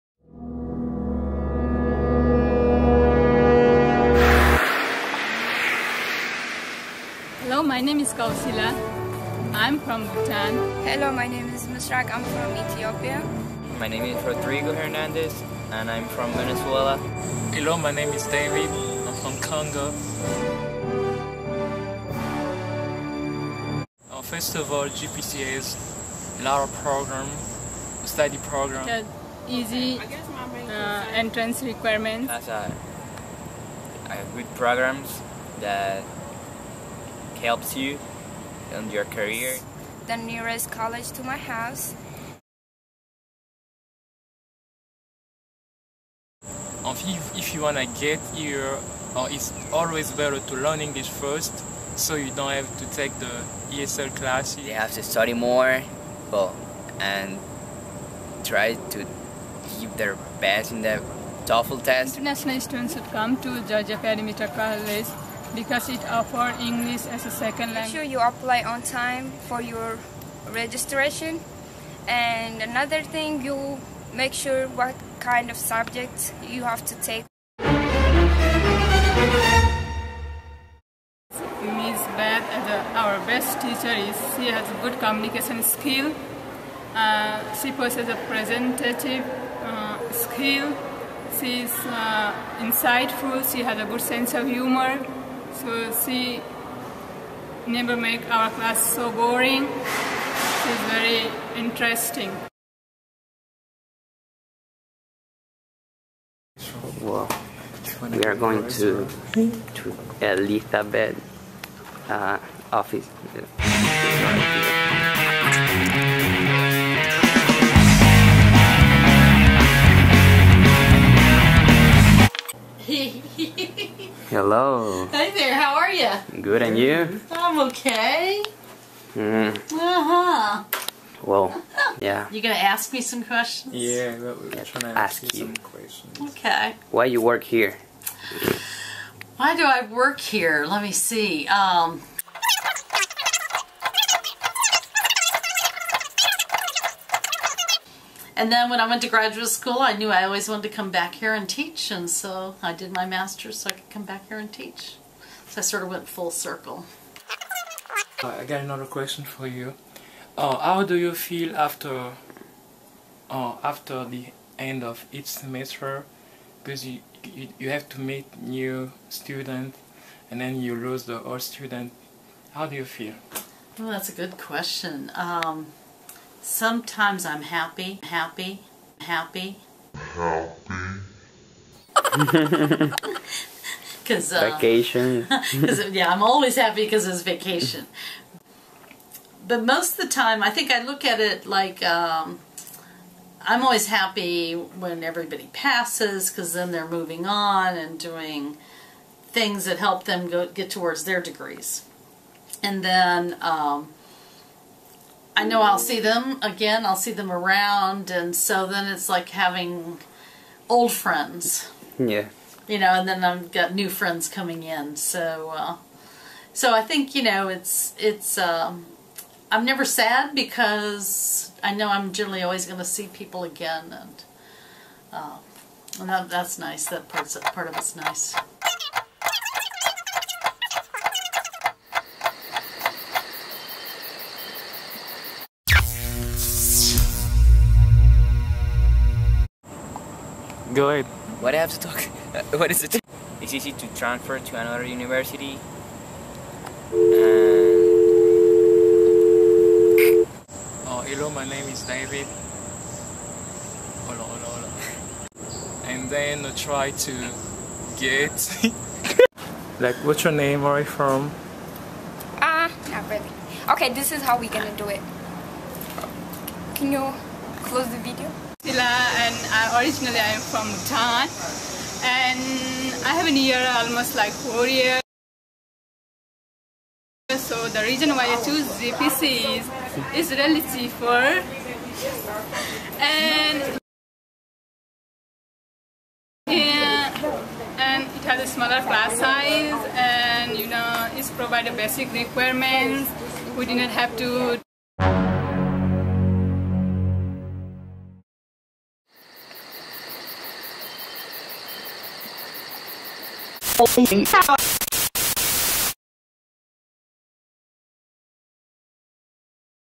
English as Second language Georgia Perimeter College
Elizabeth Wallace English as a Second language 82 class at Clarkson Campus( summer 2012).

Final project about advice to new international students who want to come at Georgia Perimeter College. This project was done by: Khari M Acharya from Bhutan, Rodrigo Jose Hernandez from Venezuela, Misrak Mekannen Sertsu form Ethiopia and David Masimbo from Congo, Democratic Republic